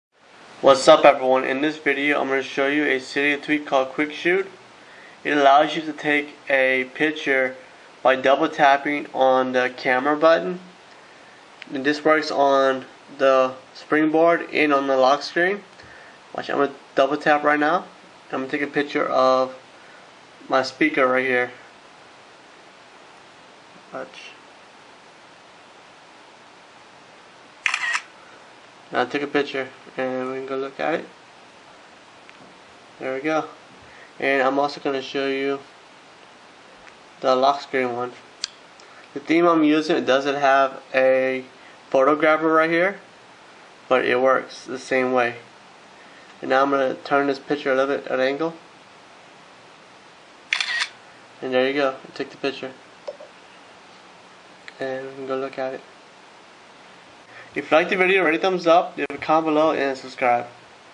QuickShoot - Snap Photos Quickly Using Double Tap [Cydia Tweak]
In this video I will show you a cydia tweak called 'QuickShoot'
It allows you to take a picture by double tapping the camera app or camera grabber.

Subcrribe to all of my channels!